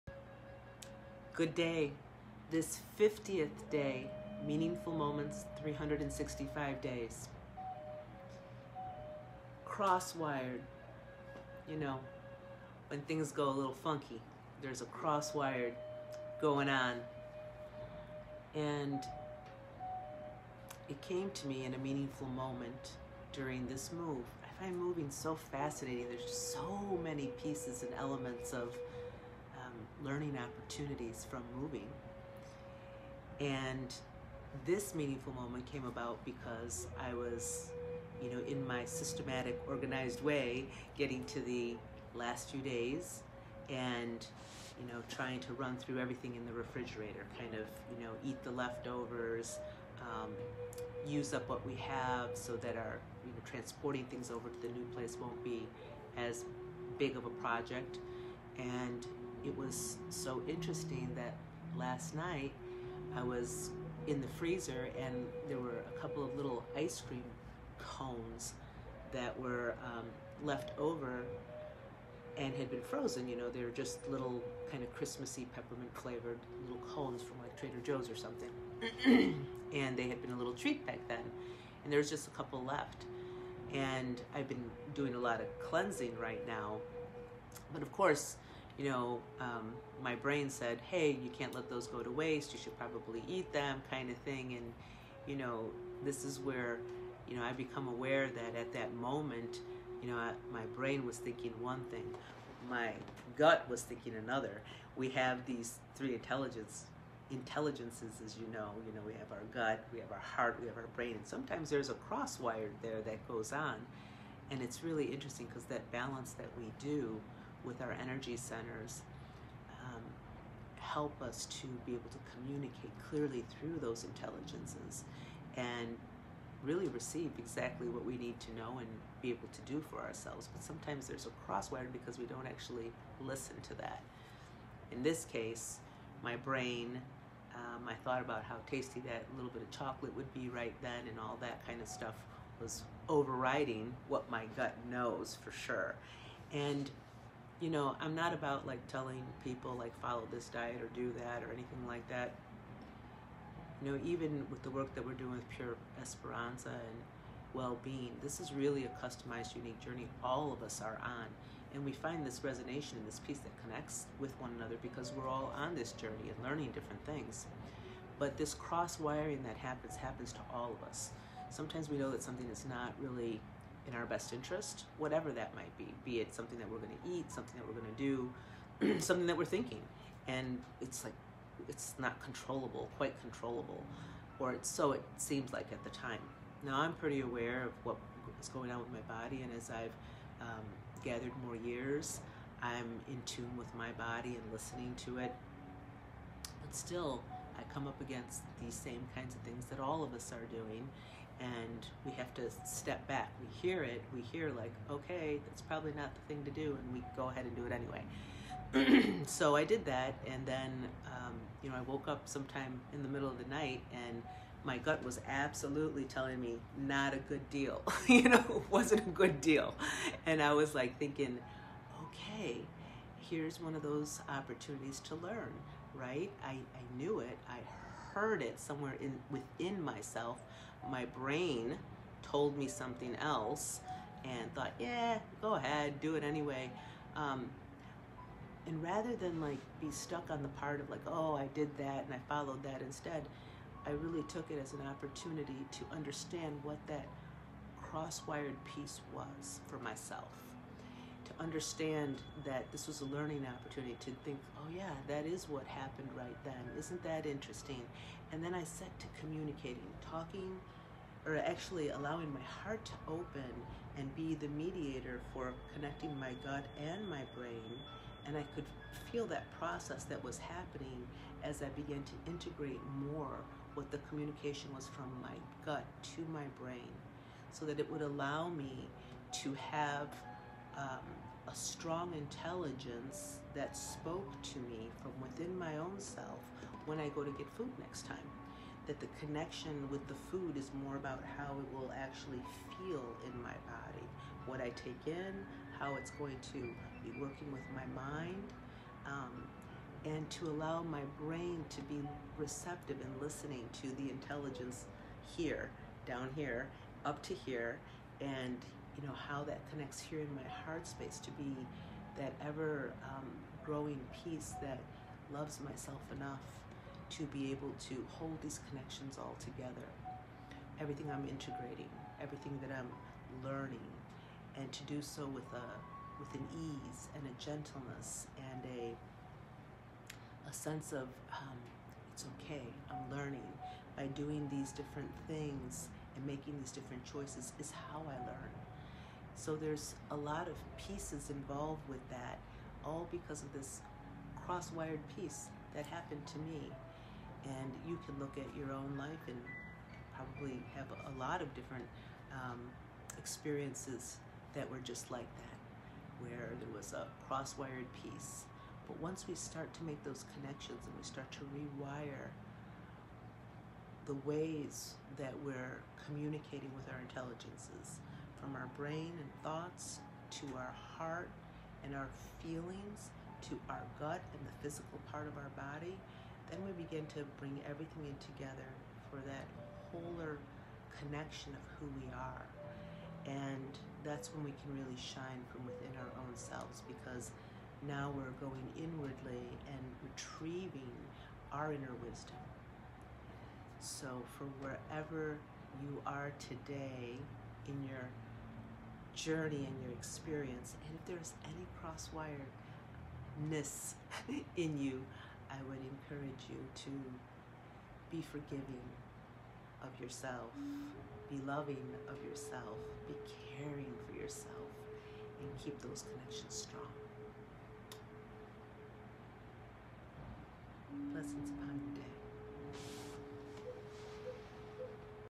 Meaningful Moments ~ 365Days, Cross-Wired
Cross-Wired, Day 50, communication between our 3 Intelligences: Gut, Heart & Brain, the learning, listening, & caring for ourSelf. 😍